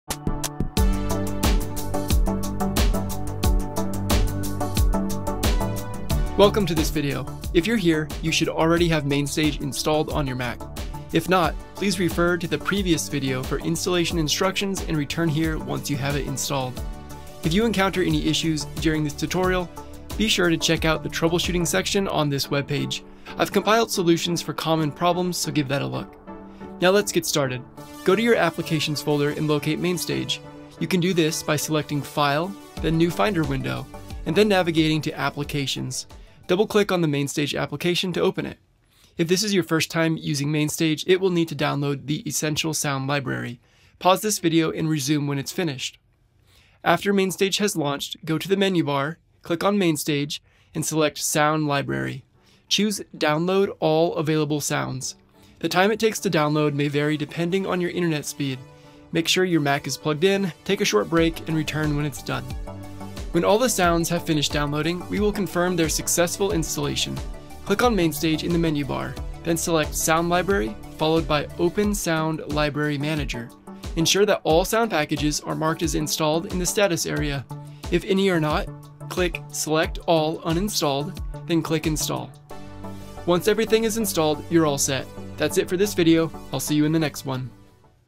Download the MainStage Sample Library | Fix Missing Sounds Errors | MainStage Tutorial
How to download the MainStage sample library.